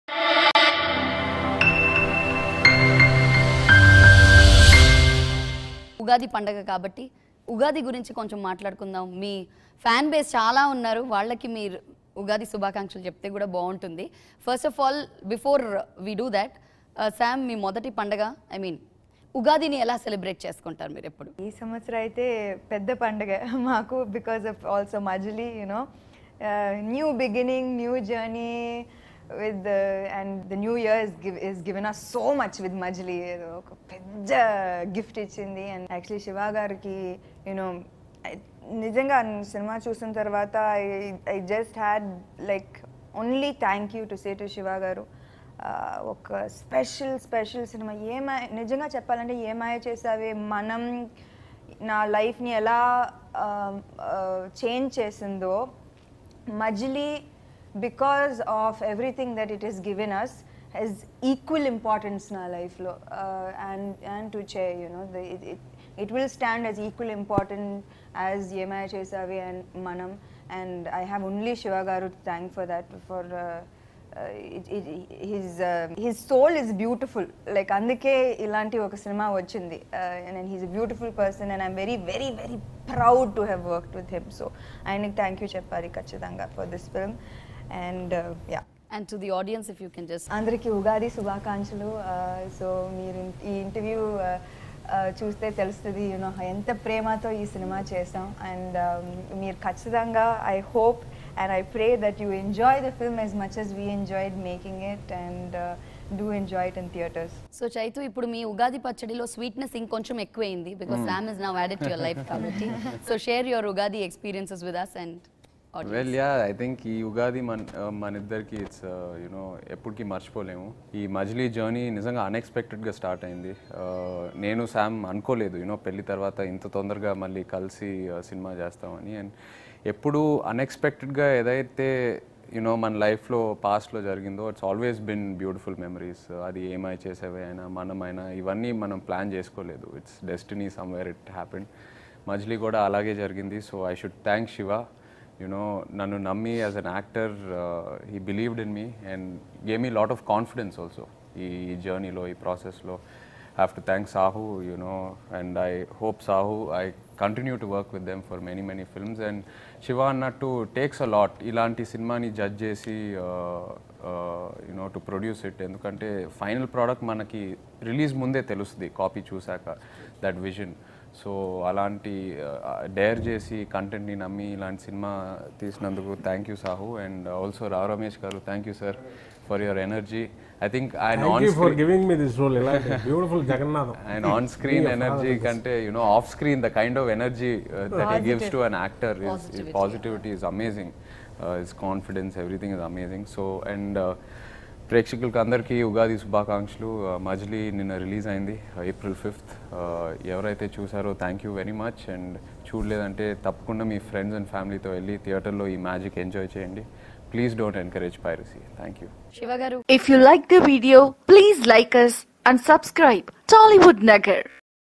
Samantha and Naga Chaitanya About Ugadi | Majili Movie Ugadi Special Interview | Tollywood Nagar
Hey guys!